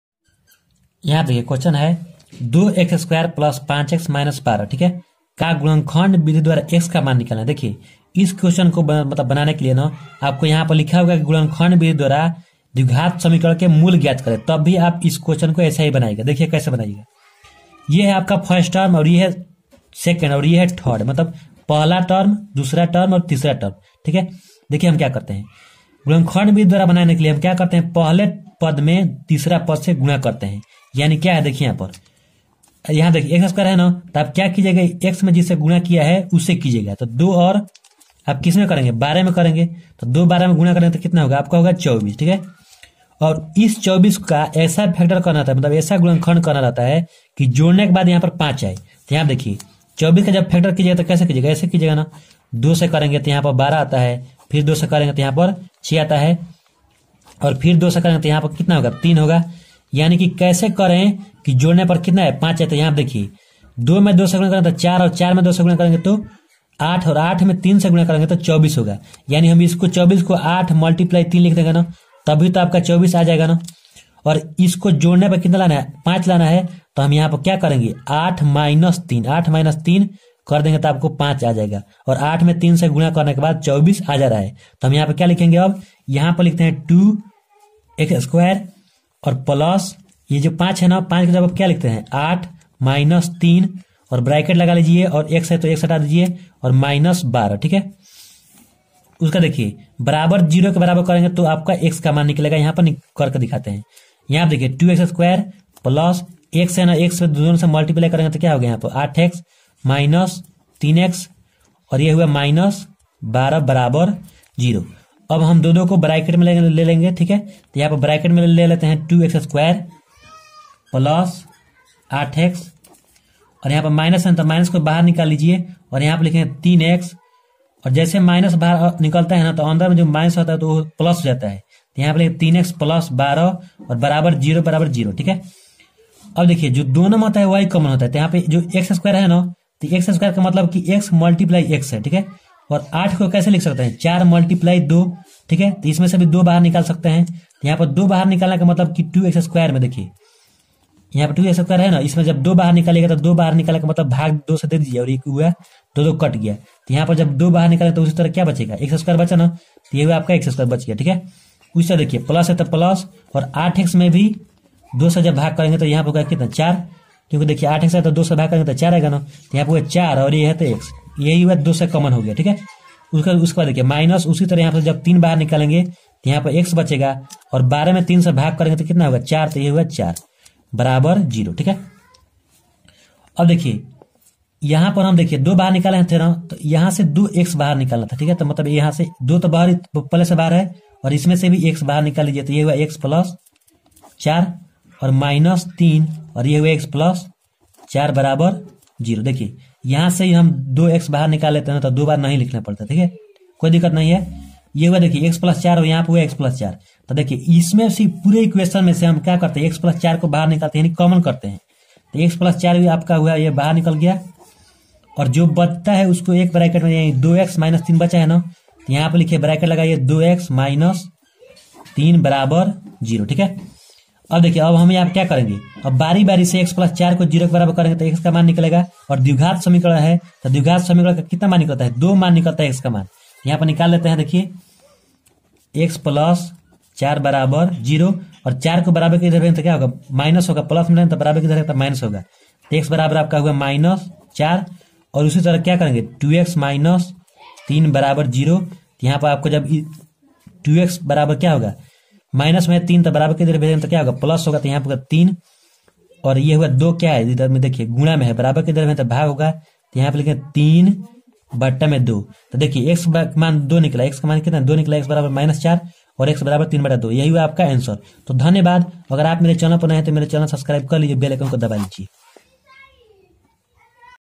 2x2+5x-12 || 2x2+5x-12=0 को गुणनखण्ड विधि द्वारा हल करें
Raviprakash Kumar
any problems comment here
WhatsApp number